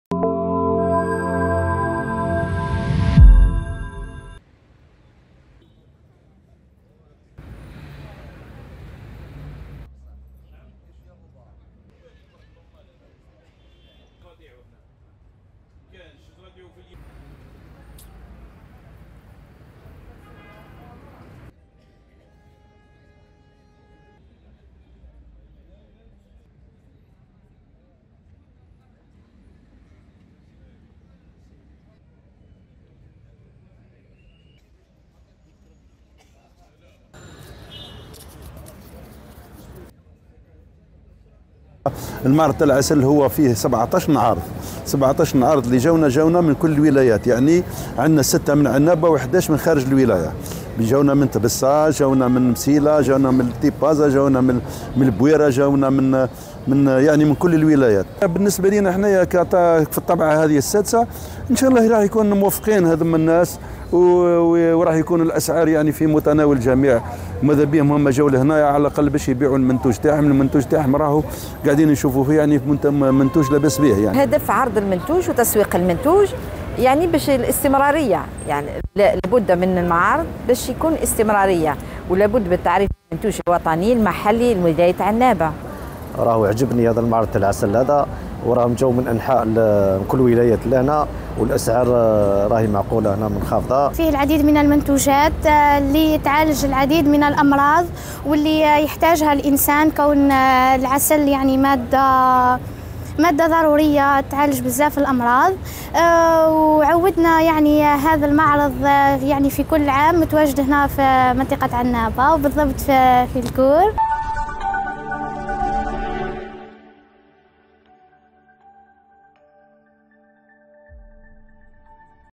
عنابة: انطلاق فعاليات الطبعة السادسة للمعرض الوطني للعسل ومنتجات الخلية
عنابة(وأج)- احتضنت ولاية عنابة، معرضا وطنيا للعسل ومنتجات الخلية، بمشاركة 17 عارضا، يمثلون مختلف ولايات الوطن، وتهدف الطبعة السادسة للمعرض، إلى تبادل الخبرات والتجارب بين المختصين في هذه الشعبة الفلاحية، بحسب منظمين، إضافة إلى عرض وبيع منتجاتهم من مختلف الأصناف